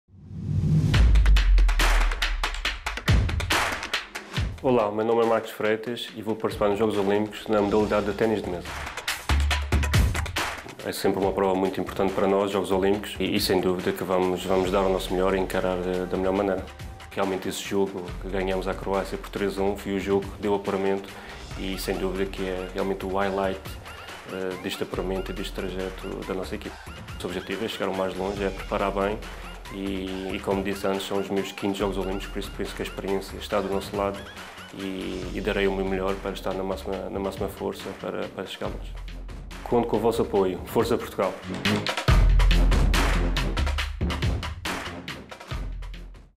Equipa Portugal Paris 2024 | MARCOS FREITAS
Conheça a EquipaPortugal 🇵🇹 para os Jogos Olímpicos Paris 2024!

Marcos Freitas entra em Paris 2024 nos seus quintos Jogos Olímpicos para competir nos torneios de singulares e equipas, este ao lado de Tiago Apolónia e João Geraldo

ℹ Leia o perfil completo em comiteolimpicoportugal.pt